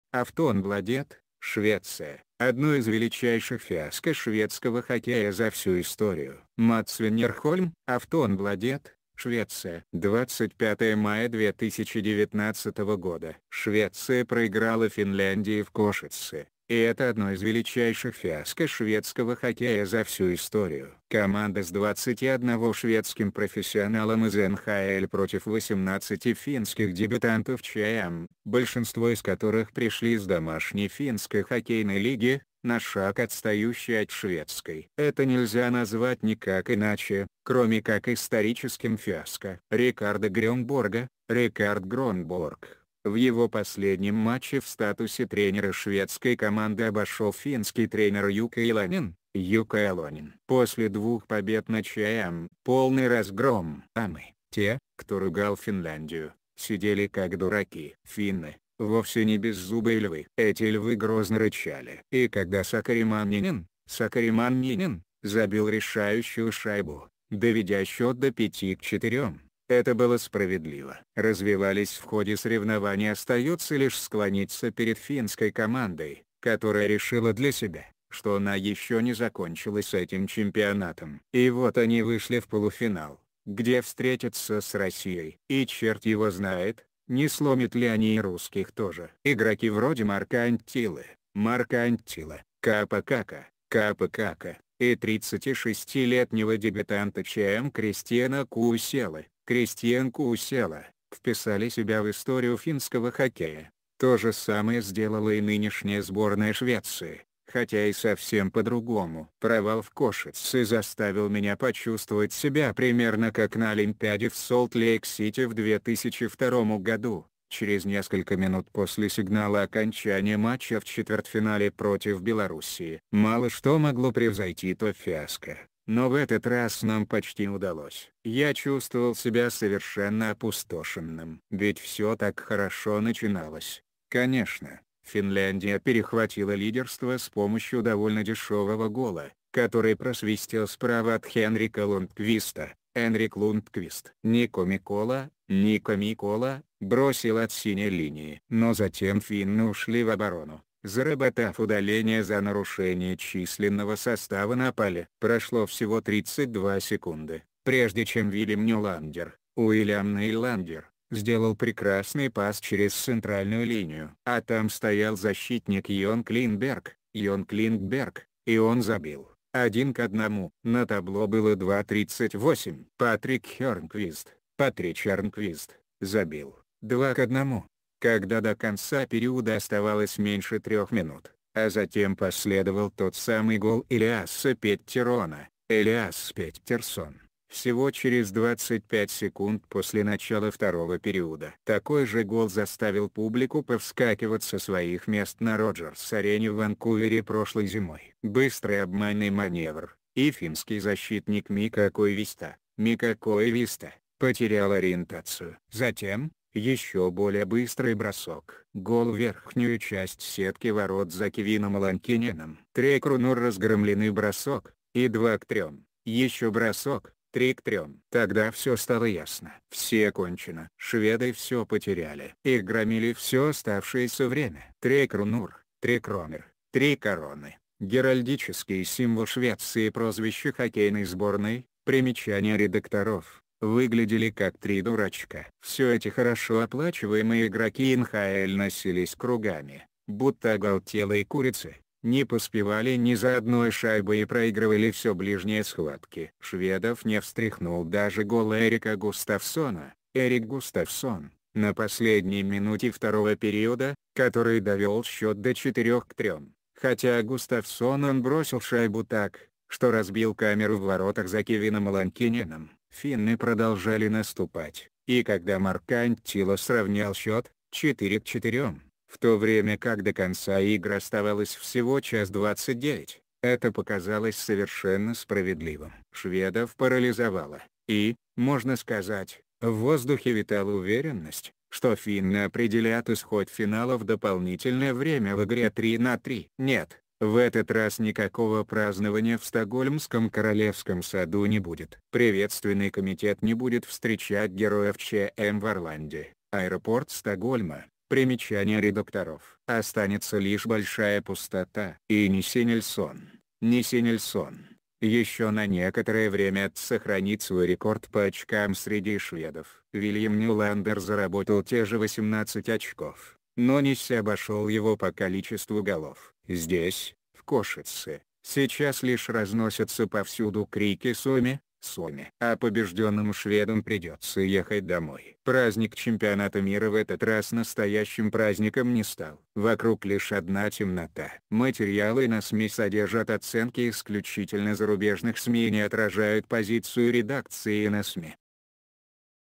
Aftonbladet (Швеция): одно из величайших фиаско шведского хоккея за всю историю.
Aftonbladet, Швеция.
Матс Веннерхольм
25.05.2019

Швеция проиграла Финляндии в Кошице, и это одно из величайших фиаско шведского хоккея за всю историю. Команда с 21 шведским профессионалом из НХЛ против 18 финских дебютантов ЧМ, большинство из которых пришли из домашней Финской хоккейной лиги, на шаг отстающей от Шведской. Это нельзя назвать никак иначе, кроме как историческим фиаско.  Рикарда Грёнборга (Rikard Grönborg) в его последнем матче в статусе тренера шведской команды обошел финский тренер Юкка Ялонен (Jukka Jalonen). После двух побед на ЧМ — полный разгром. А мы, те, кто ругал Финляндию, сидели как дураки. Финны — вовсе не беззубые львы. Эти львы грозно рычали. И когда Сакари Маннинен (Sakari Manninen) забил решающую шайбу, доведя счет до 5:4, это было справедливо.  Развивались в ходе соревнования Остается лишь склониться перед финской командой, которая решила для себя, что она еще не закончила с этим чемпионатом. И вот они вышли в полуфинал, где встретятся с Россией. И черт его знает, не сломят ли они и русских тоже! Игроки вроде Марко Анттилы (Marko Anttila), Каапо Какко (Kaapo Kakko) и 36-летнего дебютанта ЧМ Кристиана Кууселы (Kristian Kuusela) вписали себя в историю финского хоккея.  То же самое сделала и нынешняя сборная Швеции, хотя и совсем по-другому. Провал в Кошице заставил меня почувствовать себя примерно как на Олимпиаде в Солт-Лейк Сити в 2002 году — через несколько минут после сигнала окончания матча в четвертьфинале против Белоруссии. Мало что могло превзойти то фиаско, но в этот раз нам почти удало...